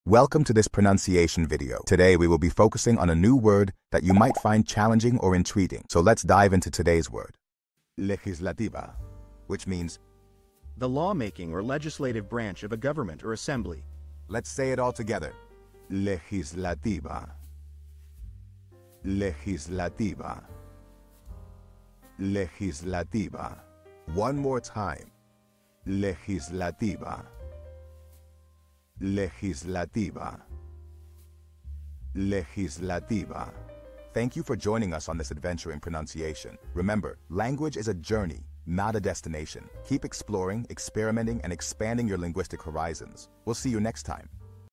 How to pronounce Legislativa
Master the Pronunciation of 'Legislativa Which Means Legislative' - which means : The Law-Making or Legislative Branch of A Government or Assembly. 📝 with @PronunciationSpanish 🎓🗣️ - [Your Guide to Spanish]

Learn to pronounce 'Legislativa' perfectly in Spanish with our easy and engaging tutorial by @PronunciationSpanish! 📖📽️ In this comprehensive video, we provide you with native speaker examples 🗣️👂 and phonetic breakdowns 🎵 to ensure you get the pronunciation just right.

How to say Legislative in Spanish